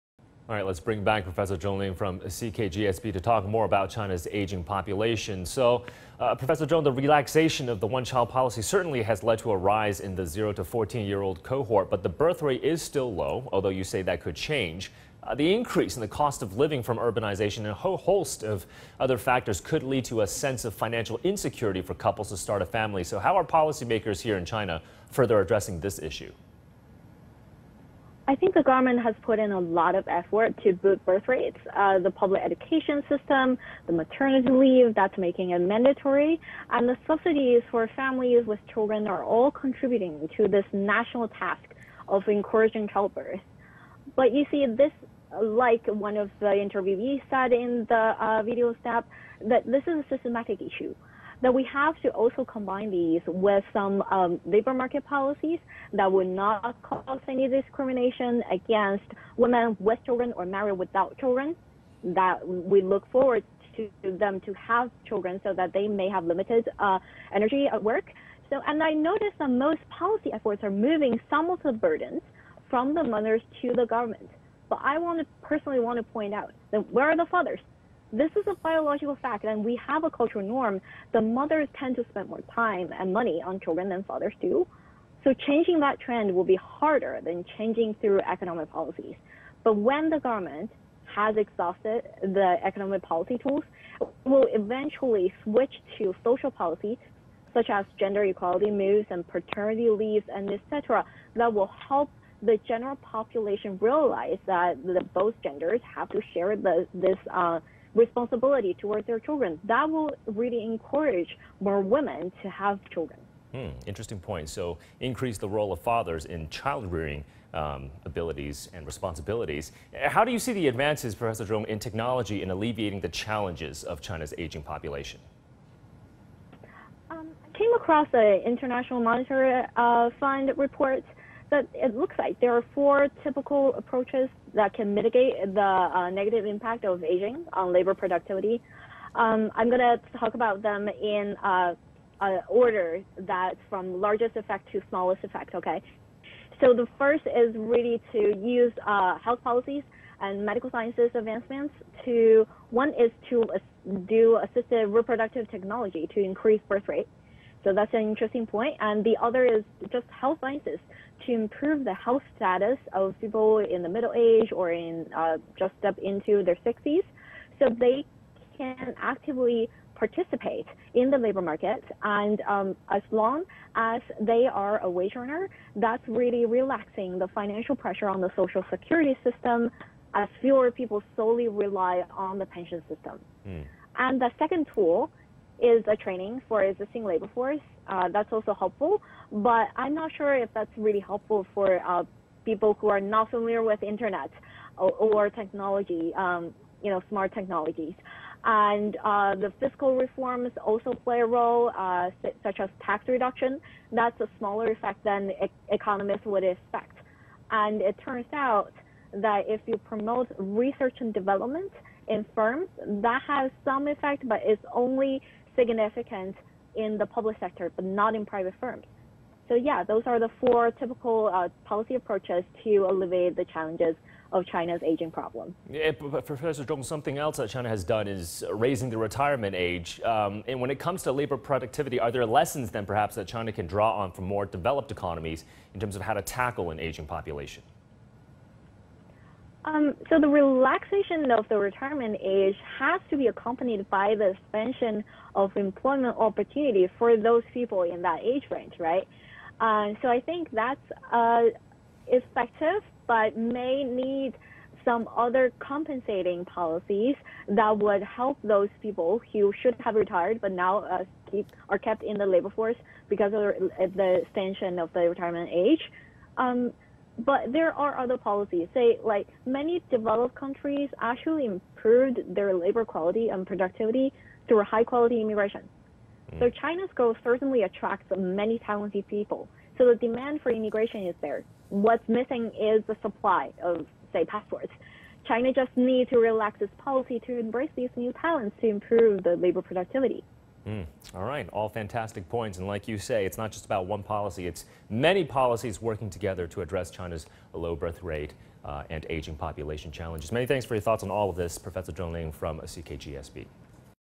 CKGSB Professor Ling Zhong: How to Alleviate the Challenges of an Aging Population in China
What problems will the aging population and slowing birthrate bring for China’s economy? How should policymakers address these issues? What lessons can China learn from developed economies in tackling the issue? Dr. Ling Zhong, Assistant Professor of Economics at Cheung Kong Graduate School of Business (CKGSB), shared her comments on CGTN.